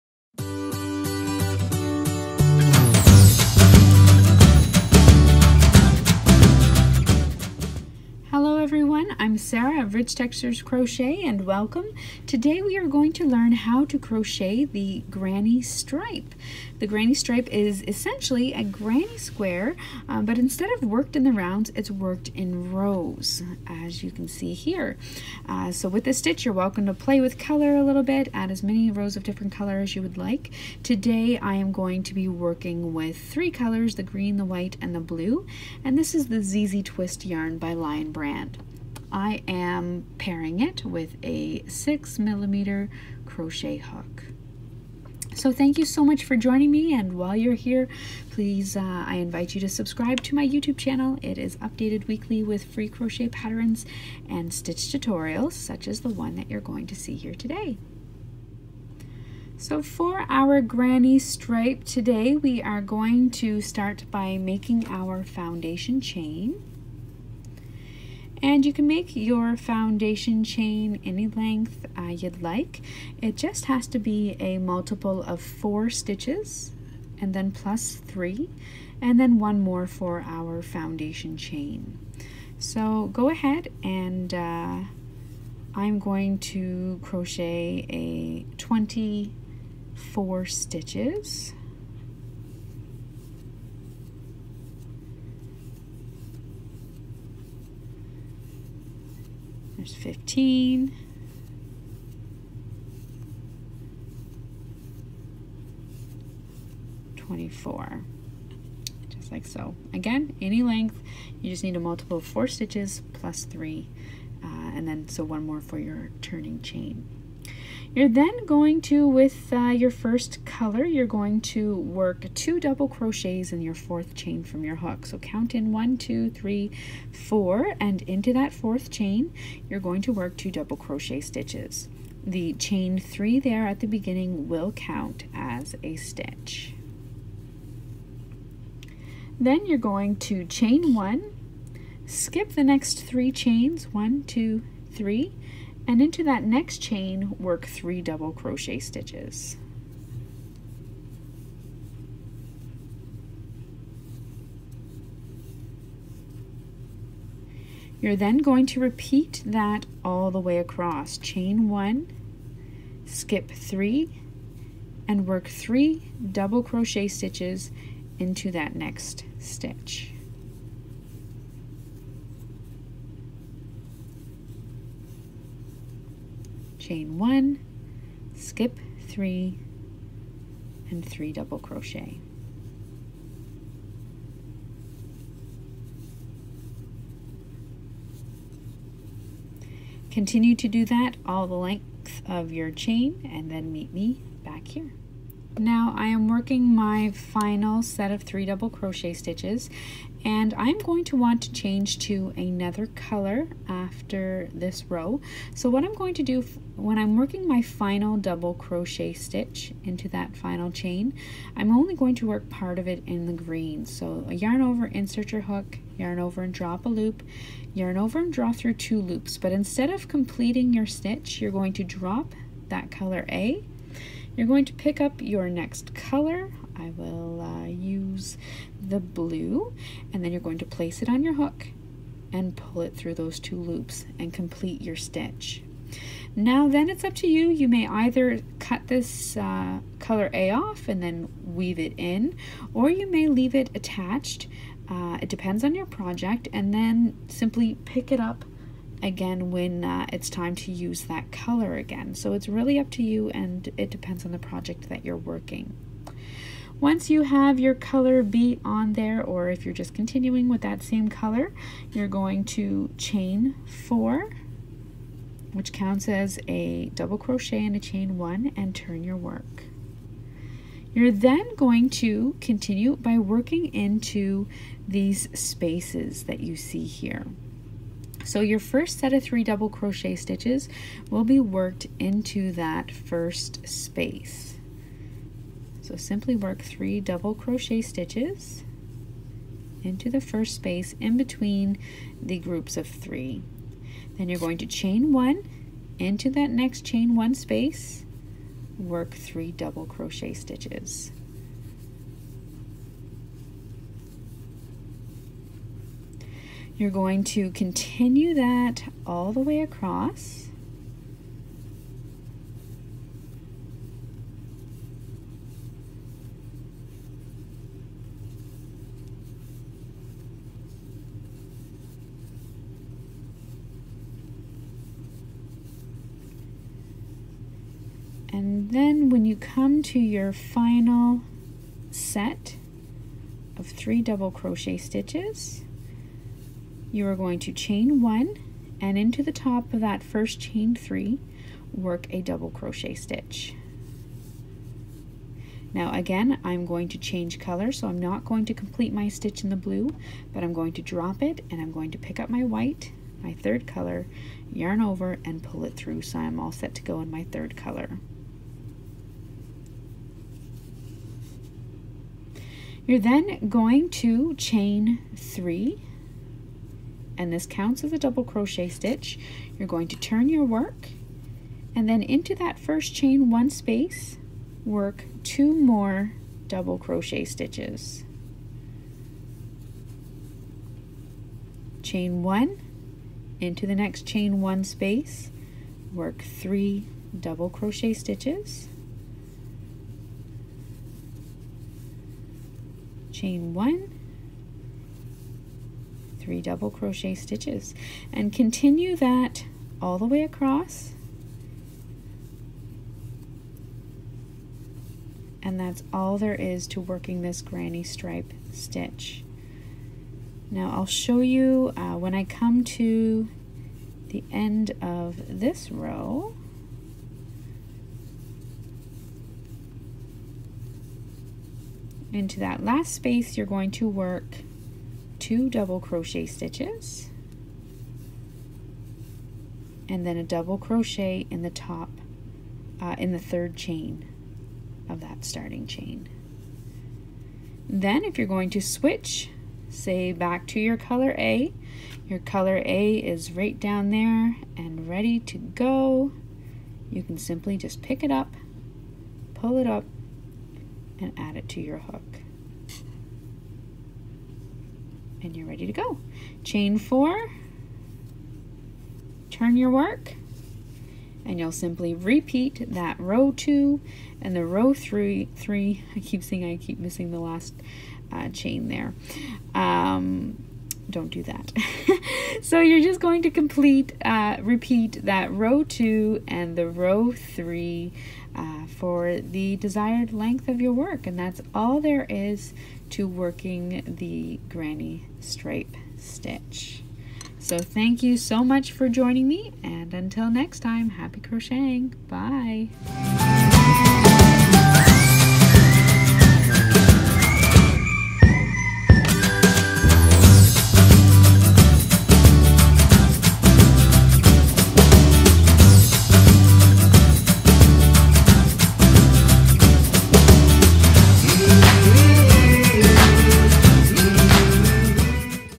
How to Crochet the Granny Stripe Stitch
Today we are going to learn how to crochet the granny stripe.  This twist on the Granny Square is worked in rows instead of in a round.  It can be worked in a single colour or multiple.

Sarah